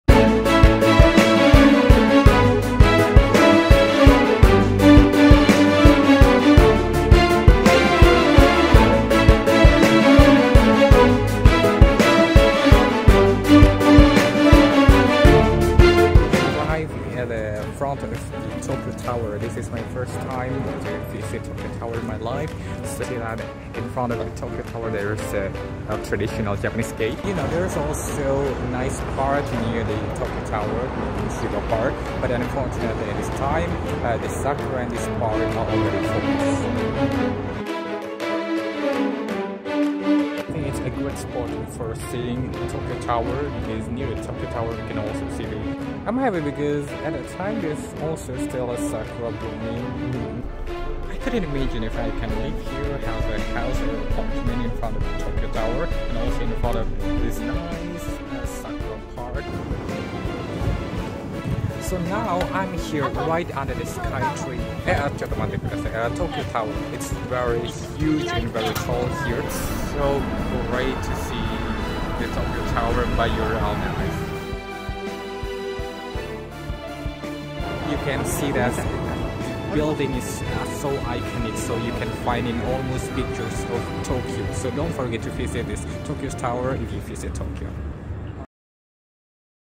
Ini alternatif menikmati Tokyo Tower dengan gratis di Jepang
Menara Tokyo (東京タワー, Tokyo Tower) adalah sebuah menara di Taman Shiba, Tokyo, Jepang. Tinggi keseluruhan 332,6 m dan merupakan bangunan menara baja tertinggi di dunia yang tegak sendiri di permukaan tanah.